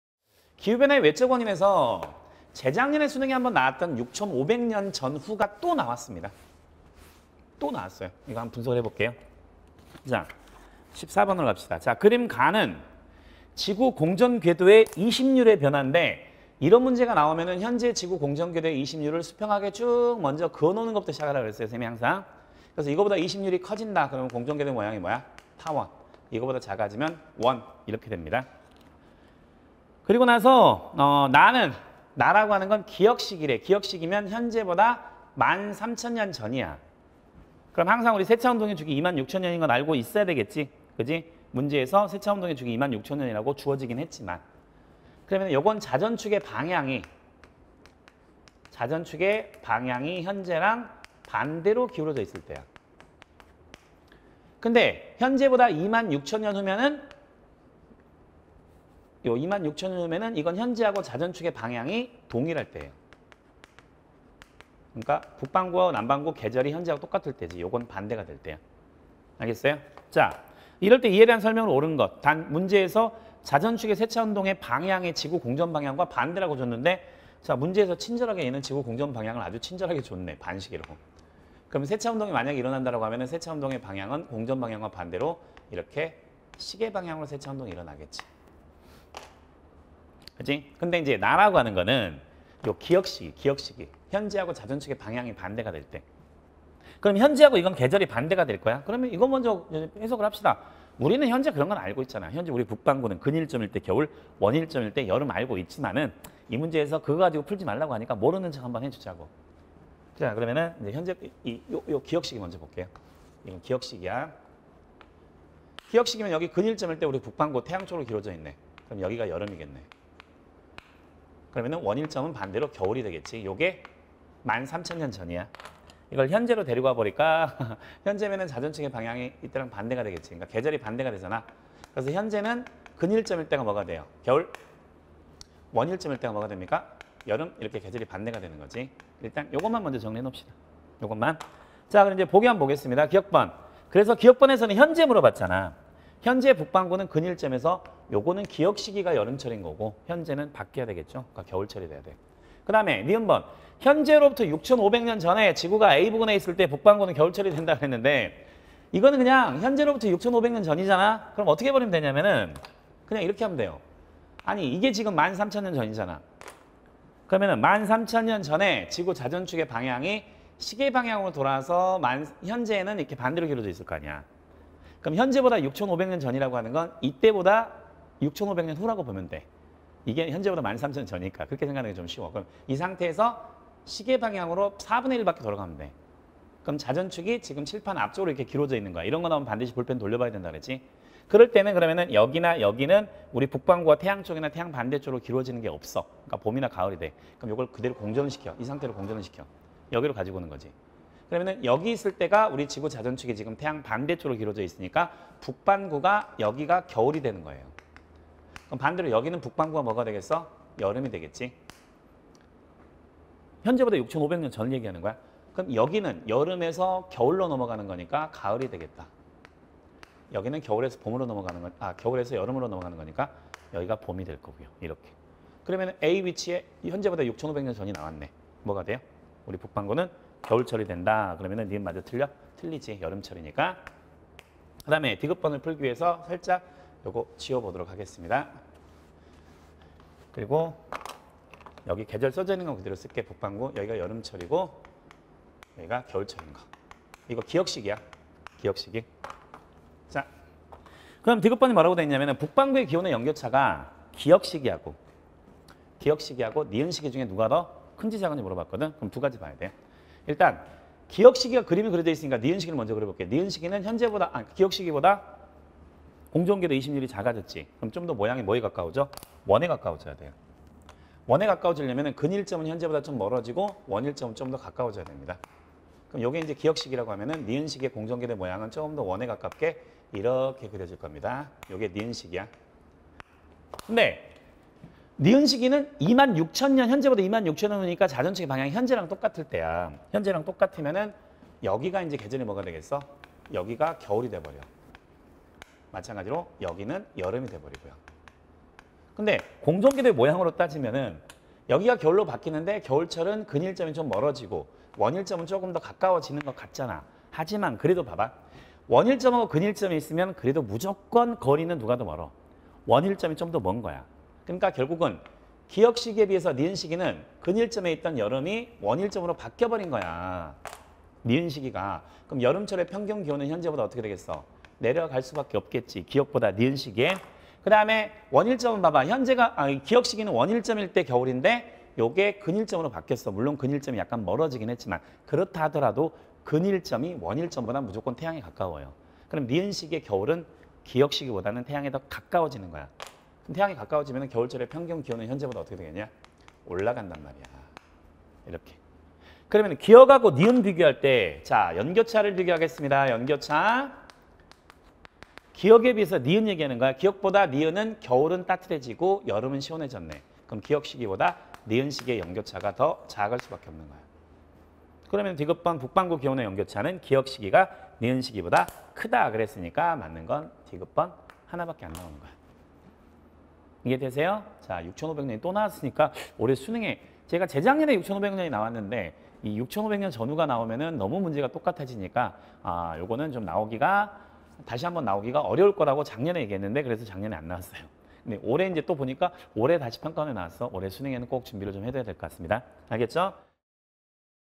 [메가스터디] 지구과학 엄영대쌤 - 9월 모평 지구과학l 해설강의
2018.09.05 시행된 고3 9월 모평 지구과학l 해설강의입니다.
전 문항 해설강의는 메가스터디에서 확인하세요!